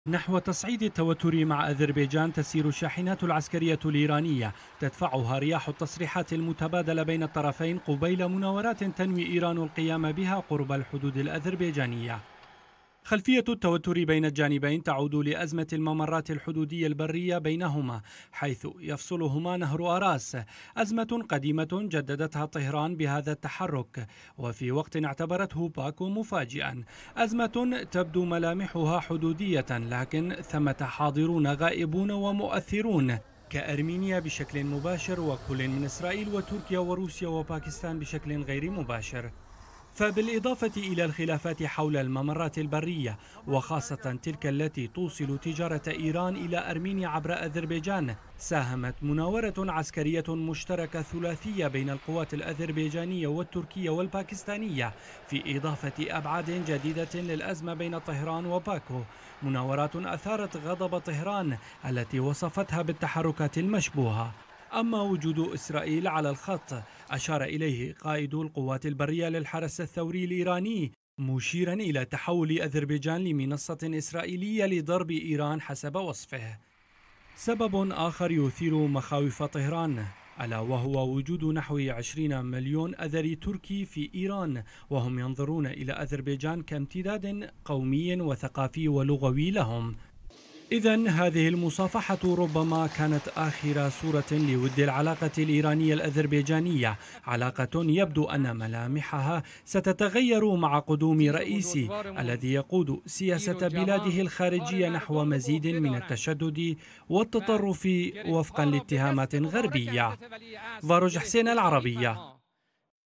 هذه حقيقة محاولة إيران التحرش عسكرياً بأذربيجان
بعد مناورة عسكرية مشتركة ثلاثية بين القوات الأذربيجانية والتركية والباكستانية.. طهران ترد بحشود عسكرية لمناورة ضخمة على حدود أذربيجان.. والسبب الخفي رسالة تهديد لأكثر من 20 مليون إيراني من أصول أذرية